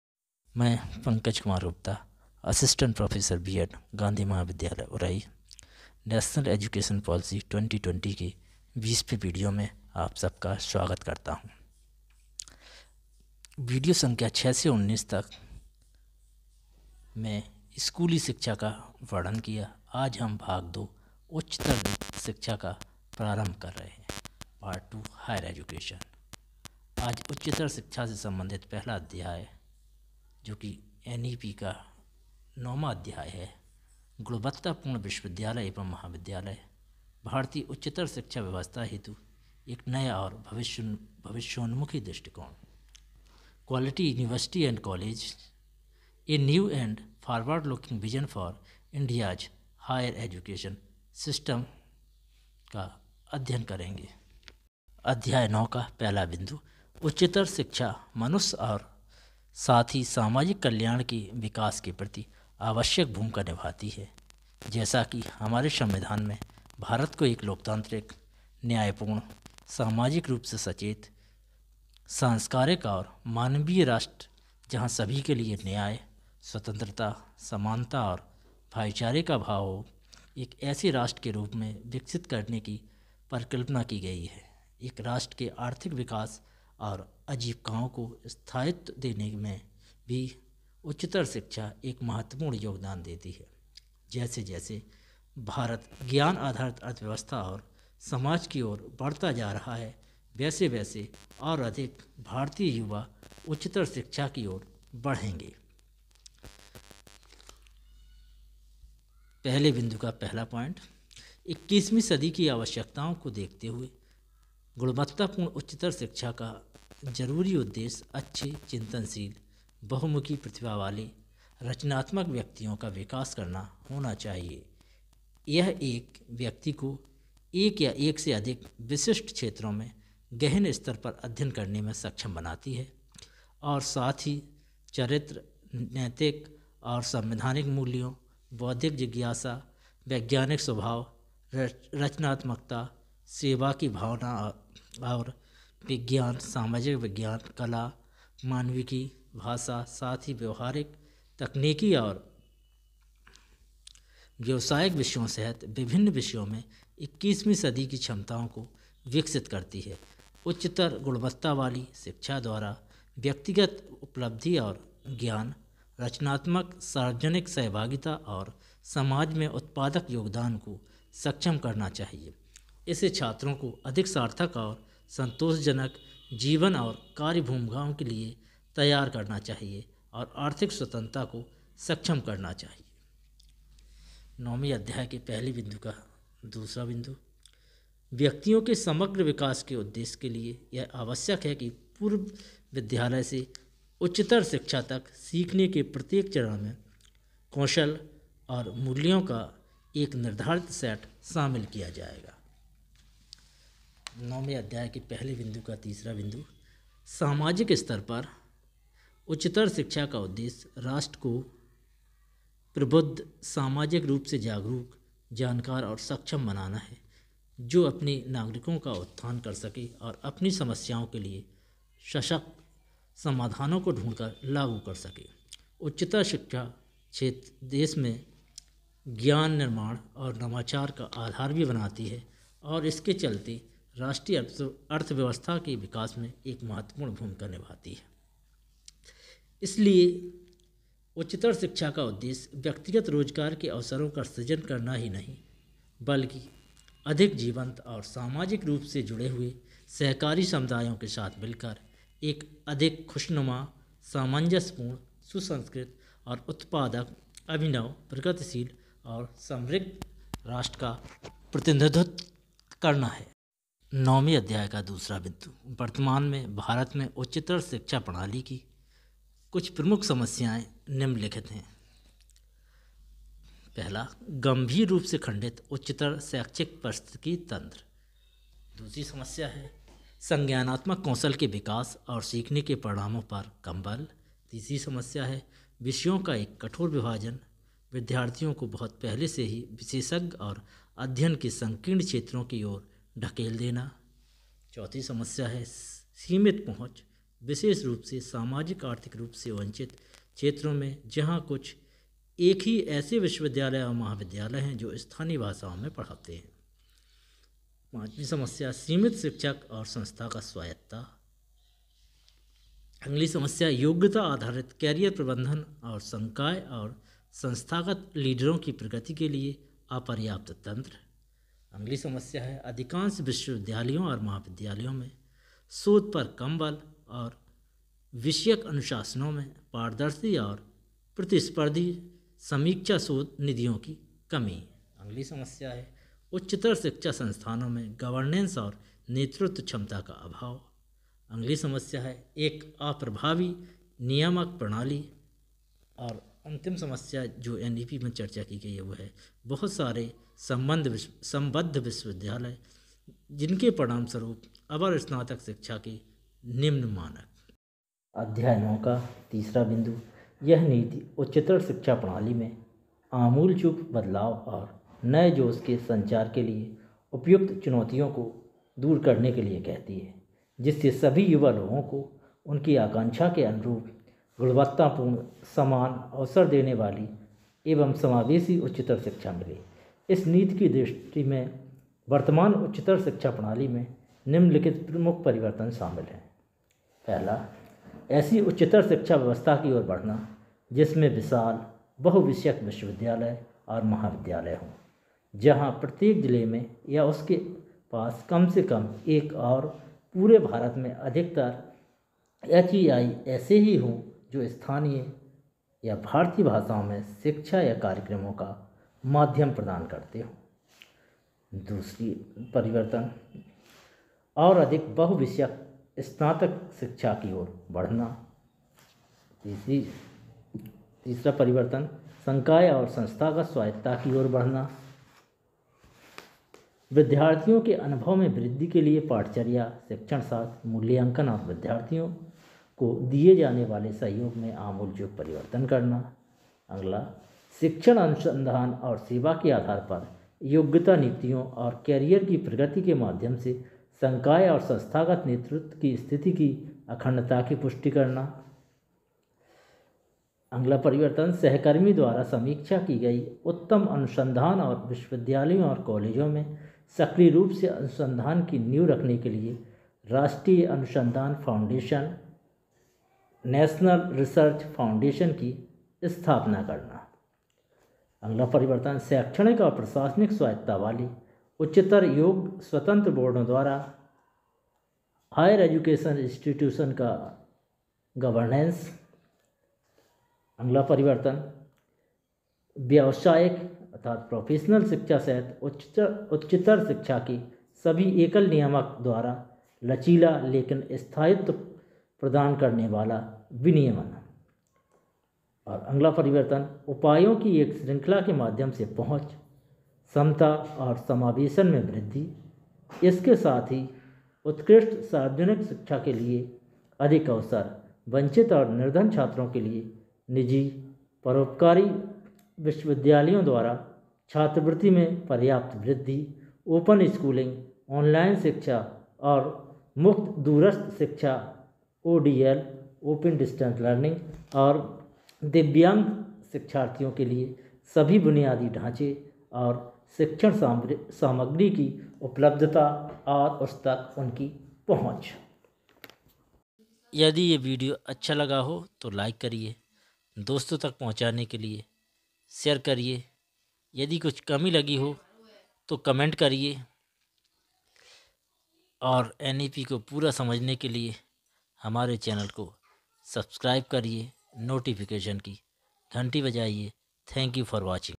NEP 2020XX:Chapter 9:Quality Universities & Colleges: A New & Forward-looking Vision for India's HES
NEP 2020 XX
भाग ॥ उच्चतर शिक्षा
अध्याय 9
गुणवत्तापूर्ण विश्वविद्यालय एवं महाविद्यालय: भारतीय उच्चतर शिक्षा व्यवस्था हेतु एक नया और भविष्योन्मुखी दृष्टिकोण
PART II. HIGHER EDUCATION
Quality Universities and Colleges: A New and Forward-looking Vision for India's Higher Education System